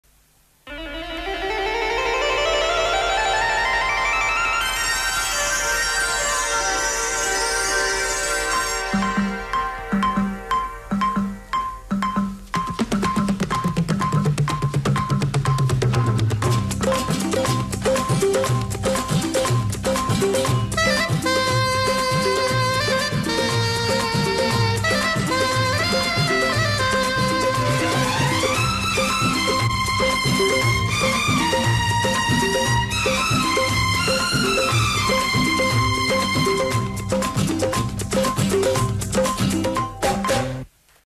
Sound Light opening intro (late 1980s, Singapore)
At first I thought this was from India. From what I heard is that it was from Singapore, not India. Plus, it has some cool Cascom Select Effects.